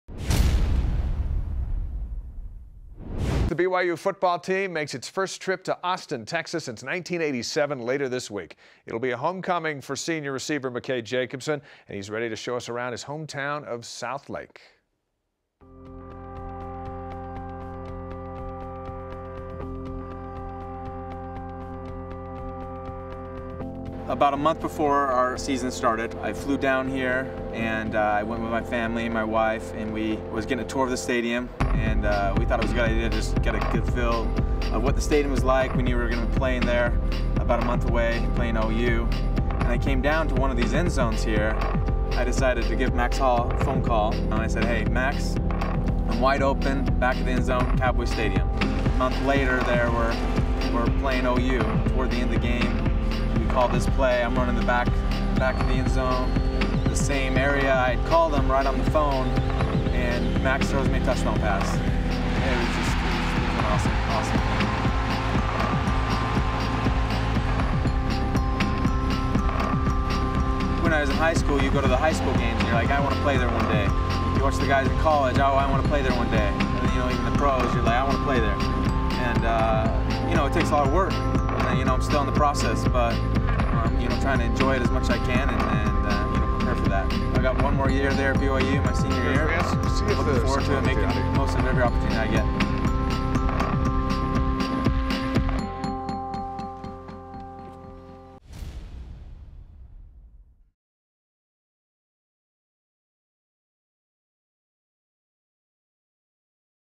McKay Jacobsen - Rise Up
Senior wide receiver and Texan McKay Jacobsen talks about playing in Texas as the team prepares to play in Austin against the Longhorns.